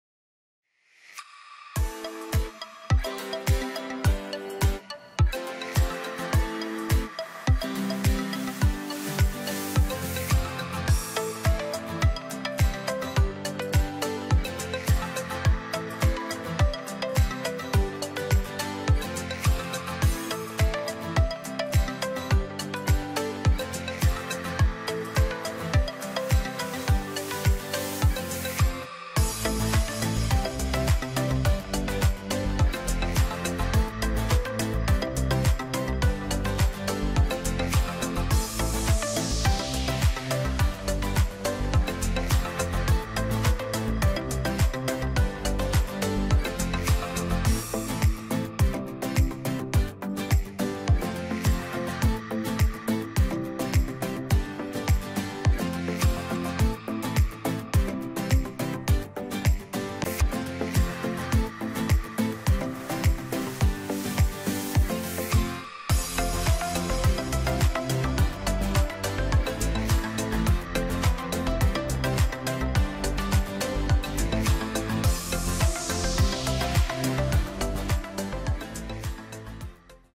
GstarCAD Express Tools- Autolayer
Is it possible to auto generate layers when drawing?

AUTOLAYER in GstarCAD creates the most needed layers as you draw.